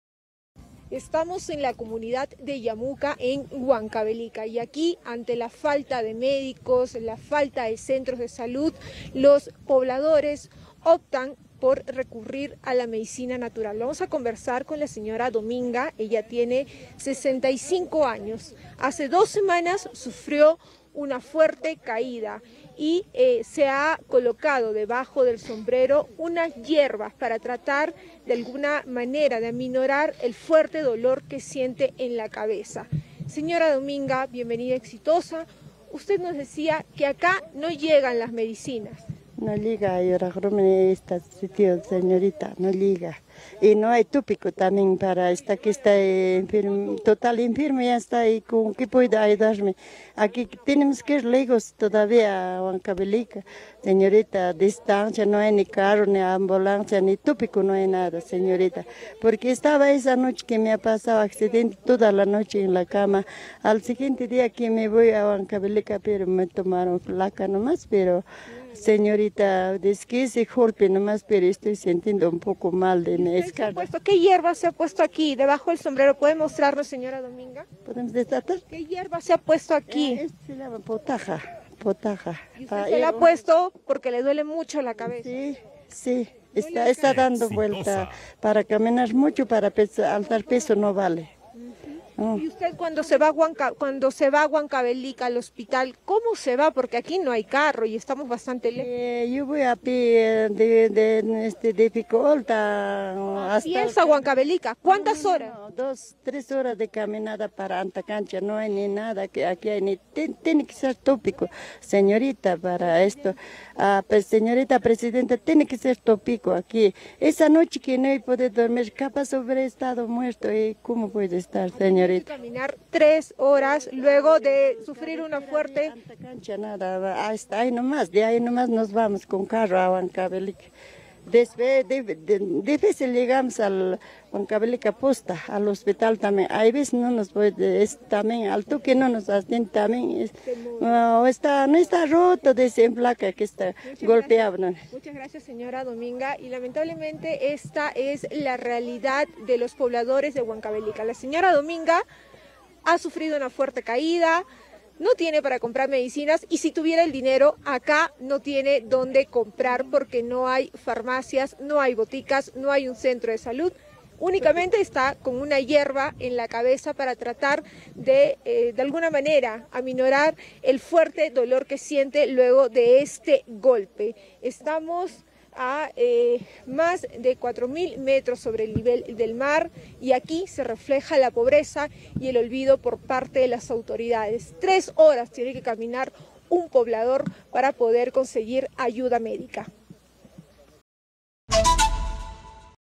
🔴🔵Anciana caminó tres horas para conseguir atención médica tras sufrir un fuerte golpe en la cabeza
En conversación con Exitosa, una adulta mayor narró que tuvo que caminar por tres horas para poder conseguir atención médica, ello luego de sufrir un fuerte golpe en la cabeza. Además, denunció la falta de medicinas en Huancavelica.

Exitosa Perú
"Exitosa: La voz que integra al Perú"

Corporación Universal